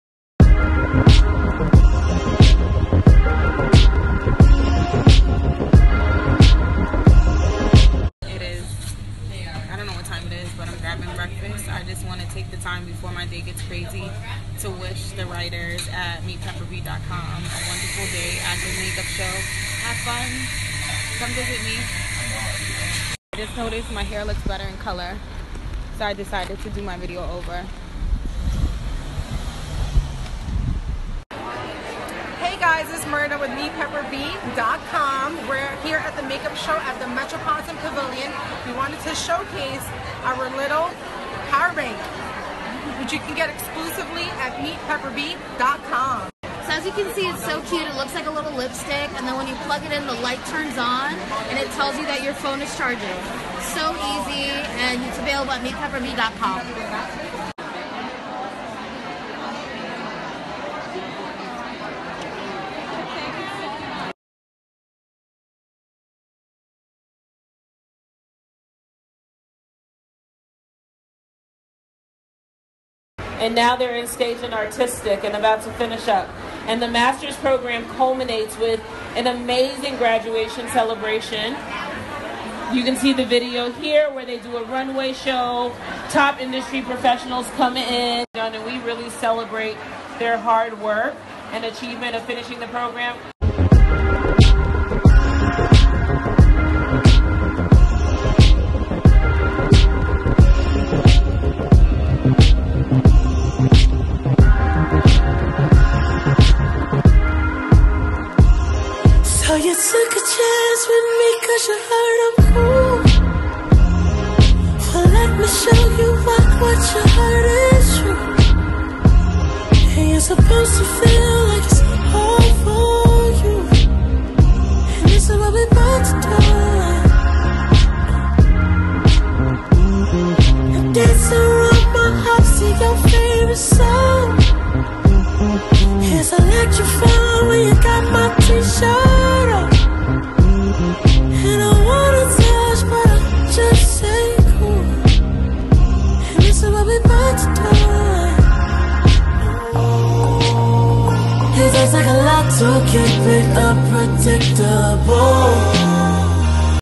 Meet Pepper B Make Up Show
The Writers at Meetpepperb.com Take over the Make-up Show Nyc